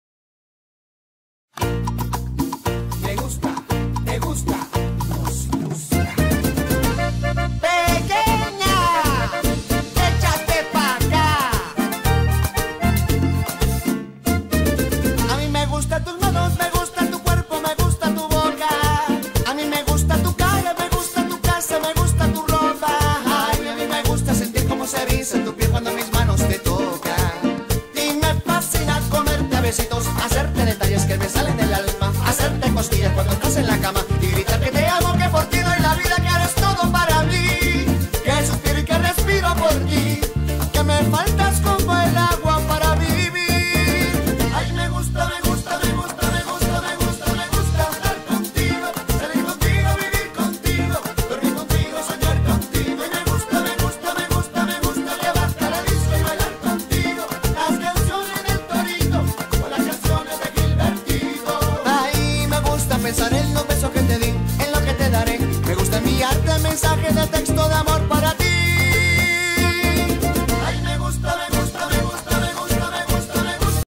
Boys.wmv
Boys showing off their Zumbatomic moves after choreography :)